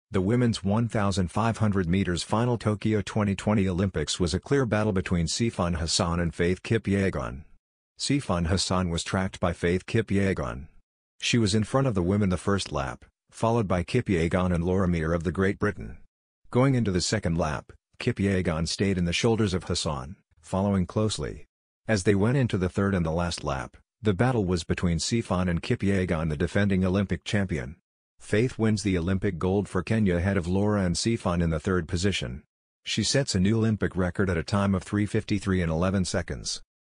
WOMEN'S 1500M FINAL TOKYO 2020 | FAITH KIPYEGON DOES IT AGAIN!!
The women's 1500m final tokyo 2020 olympics was a clear battle between Sifan Hassan and Faith Kipyegon. Sifan hassan was tracked by Faith kipyegon. She was in front of the women the first lap, followed by Kipyegon and Laura Miur of the Great Britain. Going into the second lap, Kipyegon stayed in the shoulders of Hassan, following closely. As they went into the third and the last lap, the battle was between Sifan and Kipyegon the defending Olympic champion. Faith wins the olympic gold for Kenya ahead of Laura and Sifan in the third position.  Sifan Hassan loses the battle and Kipyegon proves herself once again. It is Gold for Kenya! Congratulations to Faith Kipyegon for winning a gold medal